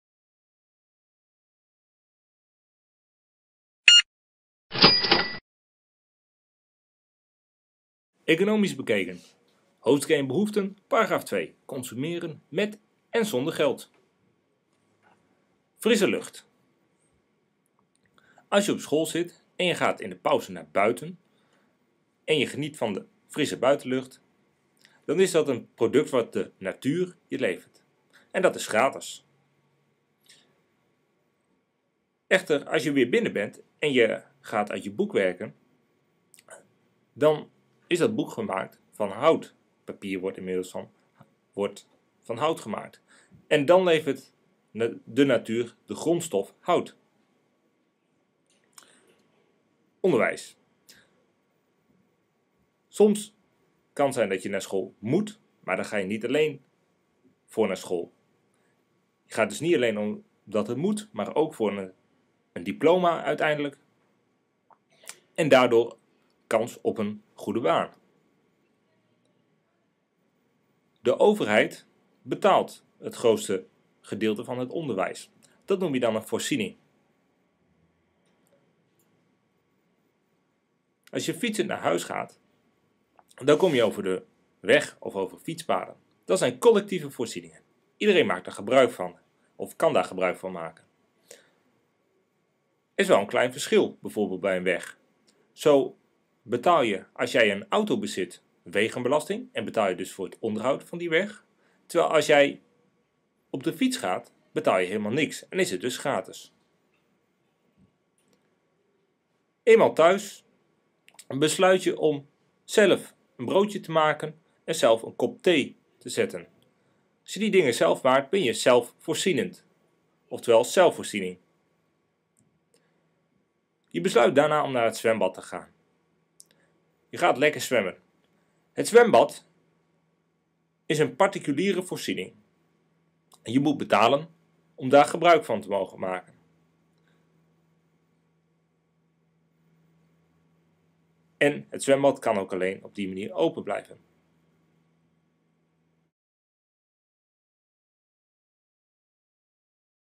Economisch bekeken BB H1 §2 Consumeren met en zonder geld
De uitleg bij de Hoofdstuk 1 van Economisch Bekeken voor BB.
§2 Consumeren met en zonder geld.